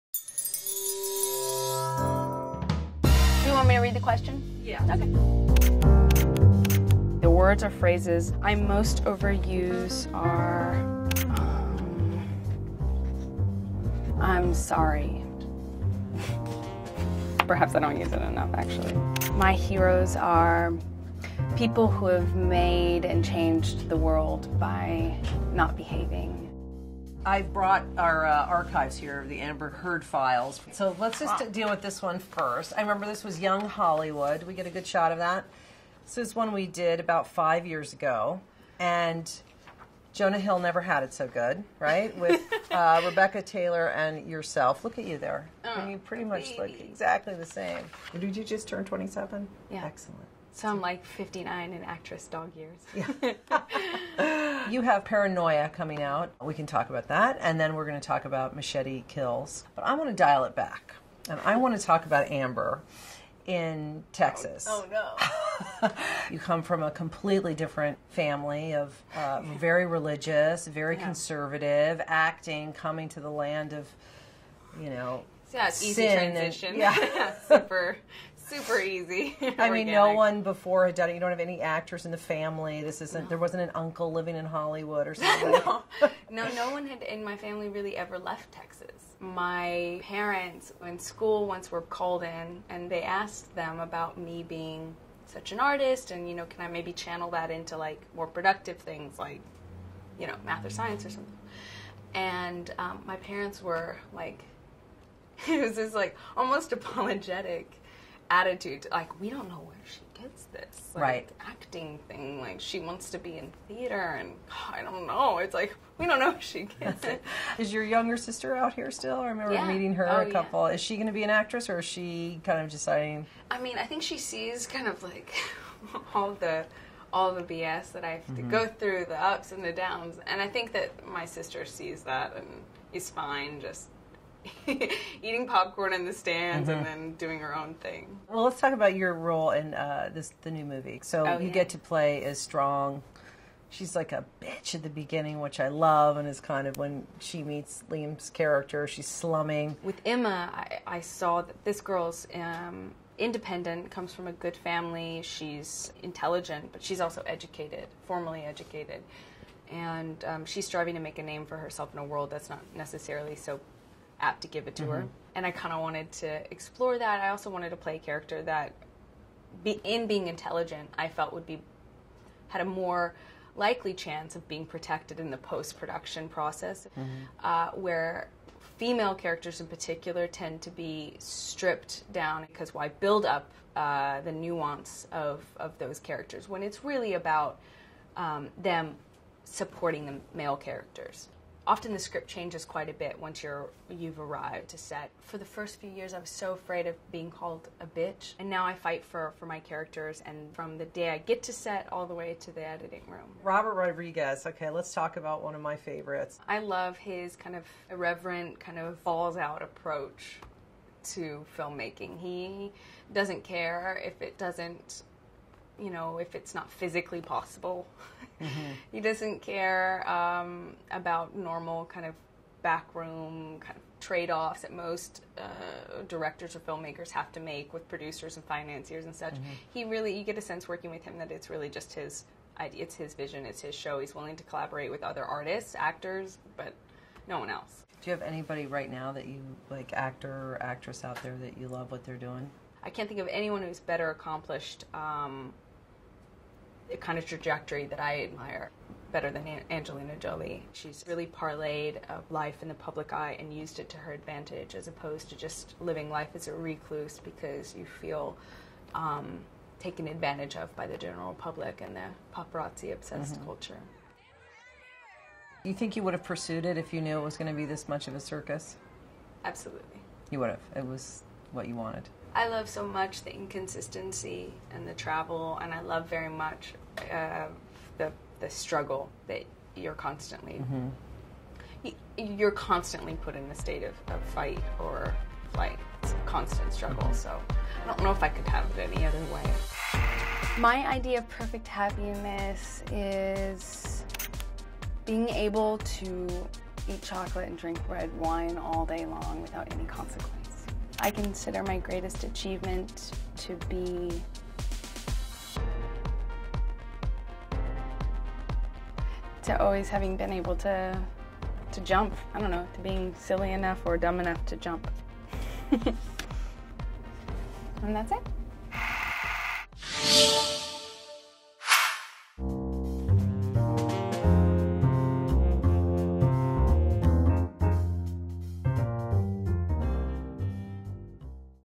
Amber Heard Answers Questions About Hollywood & Admiring Angelina Jolie | Vanity Fair
Actress Amber Heard talks to VF's Krista Smith about portraying intelligent women on-screen, her admiration for Angelina Jolie, and moving from Texas to Hollywood to pursue her acting dreams.

Shot on Canon Cinema EOS